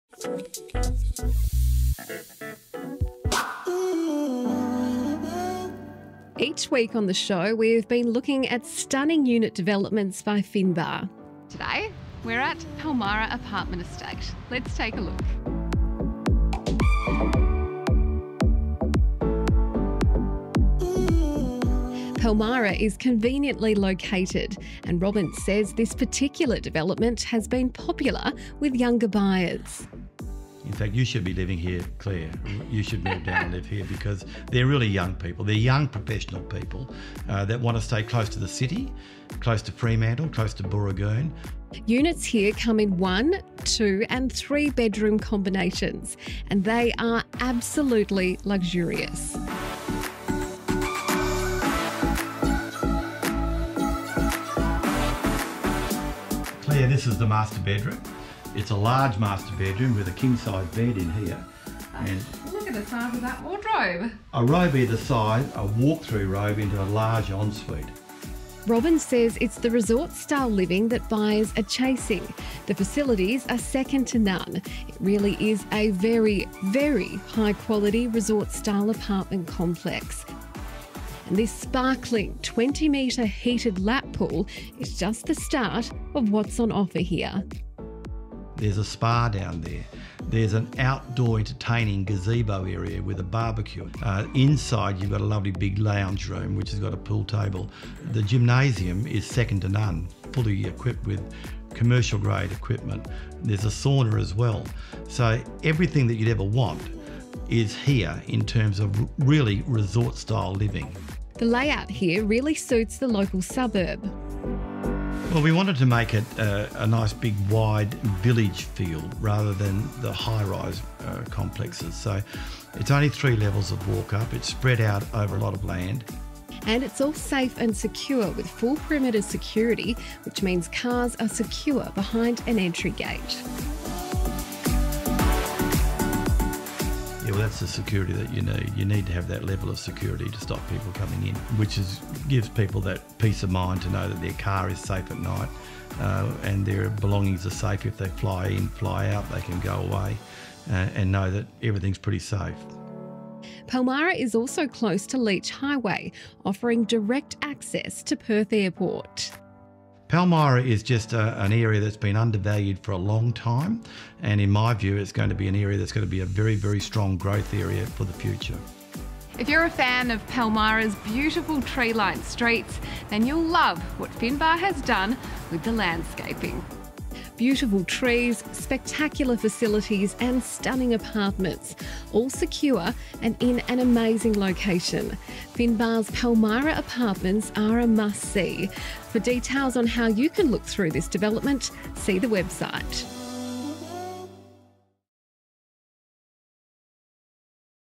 New Homes Guide | Palmyra Apartment Estate
New Homes Guide takes a closer look at Finbar's development, Palmyra Apartment Estate. Situated on the doorstep of Fremantle, these apartments place you close the city and the beach as well as local bars, cafe's and restaurants.